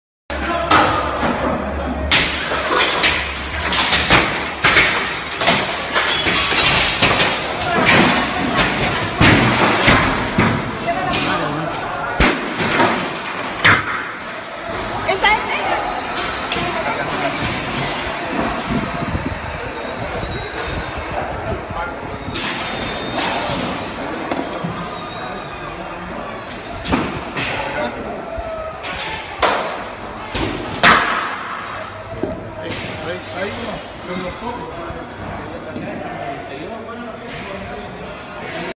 Atacan el mitico Hotel O'Higgins de Viña del Mar y provocan daños en su frontis y dependencias
Atacan el mitico Hotel O'Higgins de Viña del Mar y provocan daños en su frontis y dependencias del recinto.